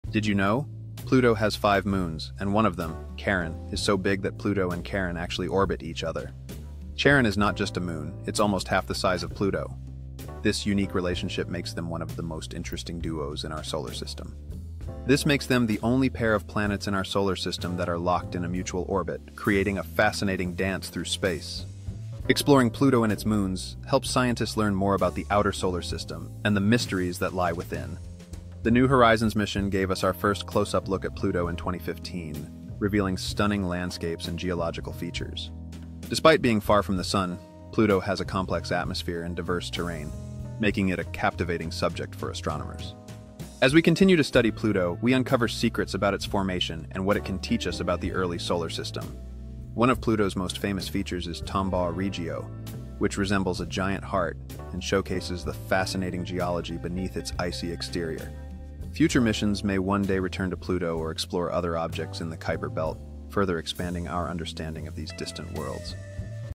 Did You Know? – Pluto has five moons
Discover the fascinating relationship between Pluto and its moon Charon, where they orbit each other in a unique cosmic dance. This video showcases stunning visuals of both celestial bodies, highlighting Charon's significant size compared to Pluto.

Explore the intricate orbits of Pluto's five moons and learn about the groundbreaking discoveries made by the New Horizons mission. From icy landscapes to complex atmospheres, Pluto continues to captivate astronomers and reveal secrets about our solar system's formation.

Join us on this journey through space and uncover the mysteries of the outer solar system! Pluto Charon NewHorizons SpaceExploration Astronomy.